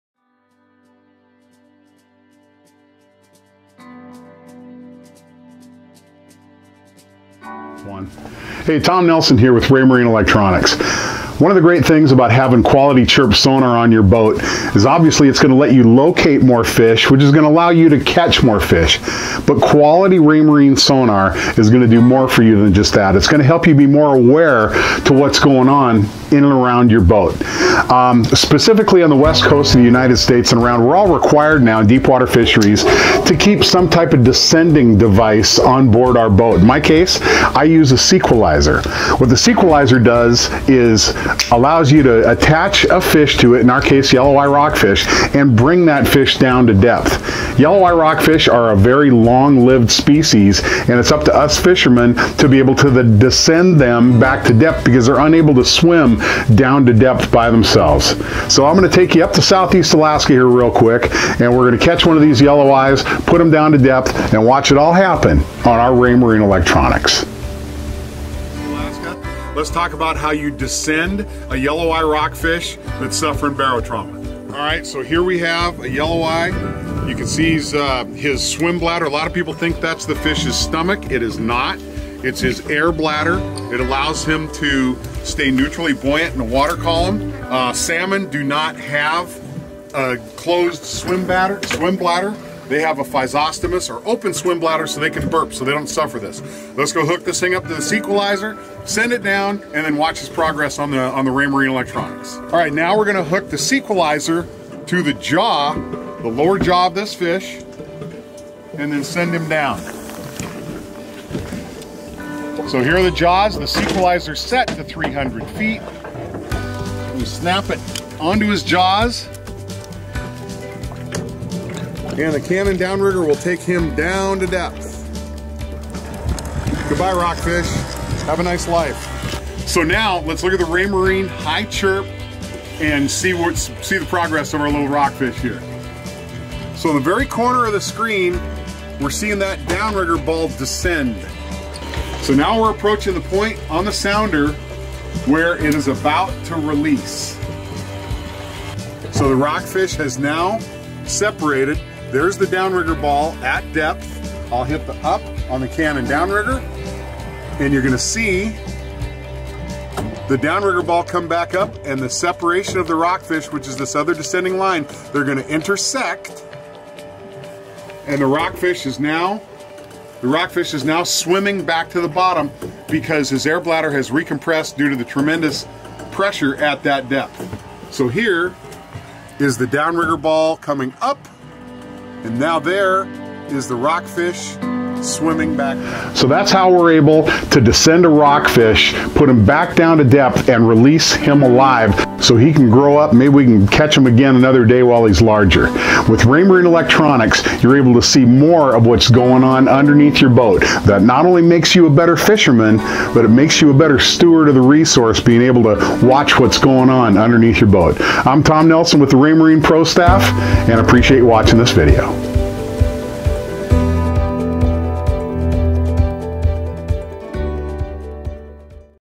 Raymarine Chirp Sonar: Monitoring a successful rockfish descent
The Ourdoor Line's Tom Nelson takes you to Southeast Alaska and shows you how to successfully descend a yelloweye rockfish. For a released yelloweye to survive, it must be returned to the deep water. It's swimbladder will not allow it to swim back down unassisted.